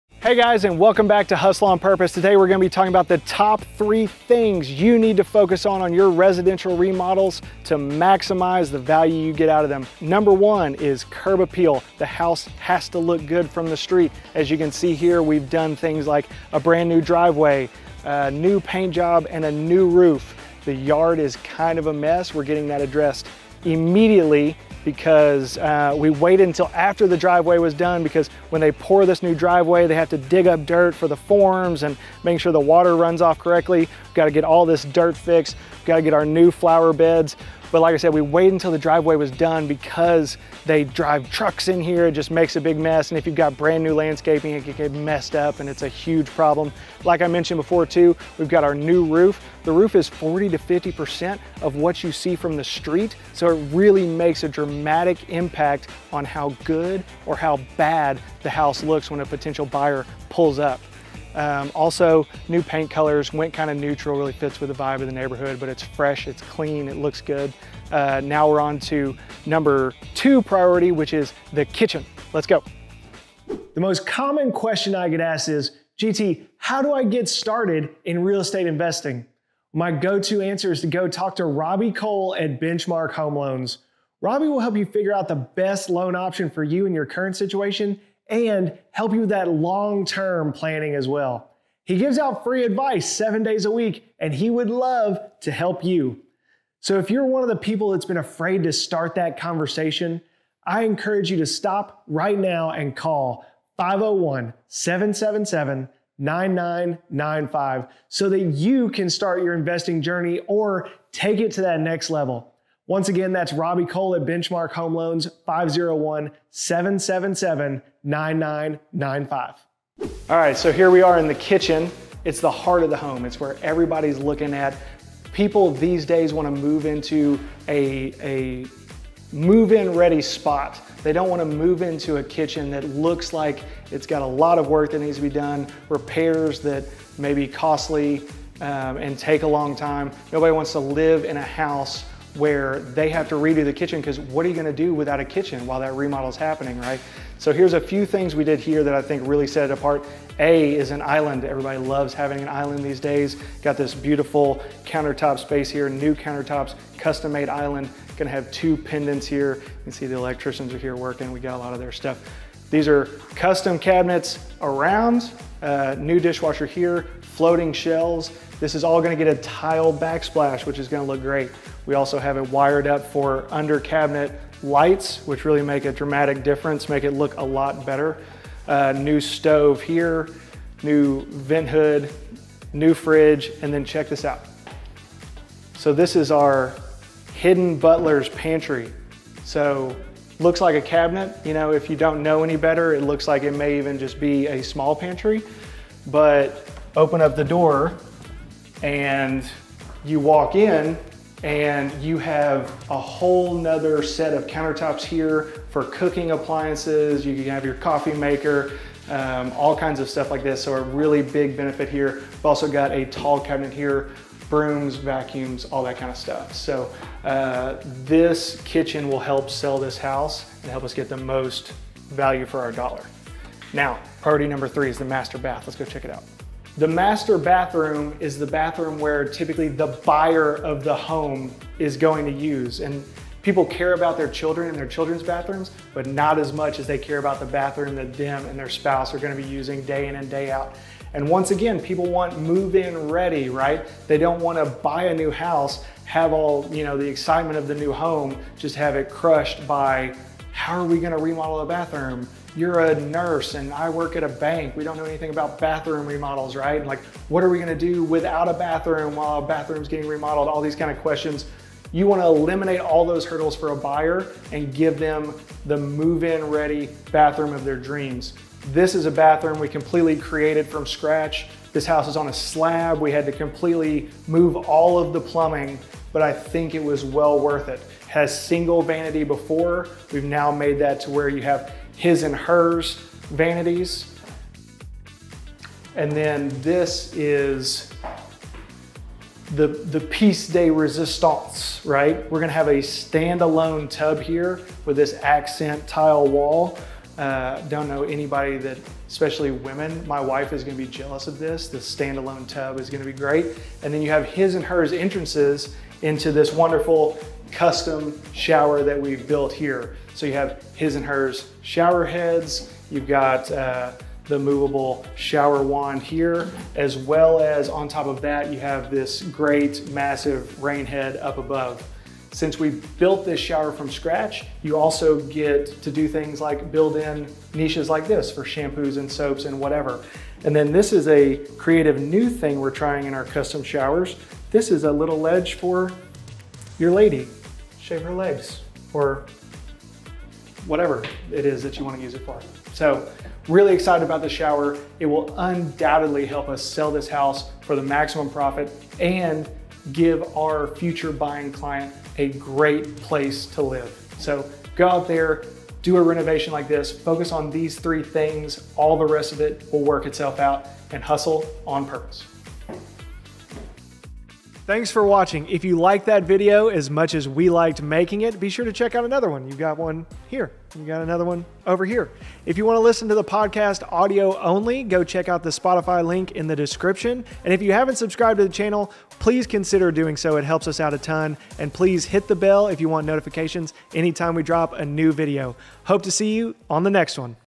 Three Tips To Maximize Profit On Your Next Flip House
Are you ready to take your real estate flipping game to the next level? In this video, we're sharing the insider secrets you need to maximize your profits on your next flip house.

We'll walk you through real-world examples and practical advice that you can apply to your next project immediately. Whether you're a seasoned pro or just dipping your toes into the world of real estate investment, these tips are designed to help you navigate the market with confidence and precision.

Benchmark Home Loans